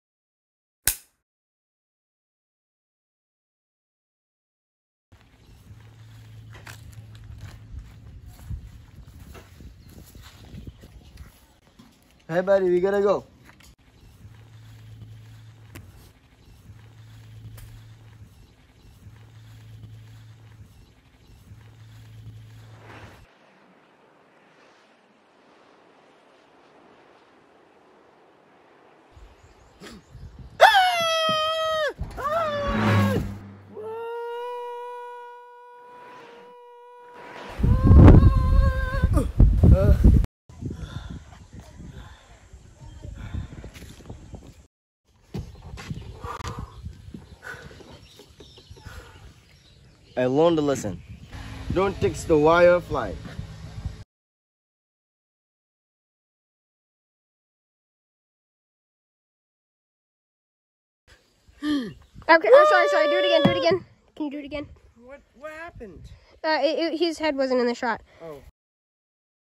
The Flying Broom 2
Ivan flies away on his broom again, but the phone he is texting on distracts him from flying right. Will he learn his lesson?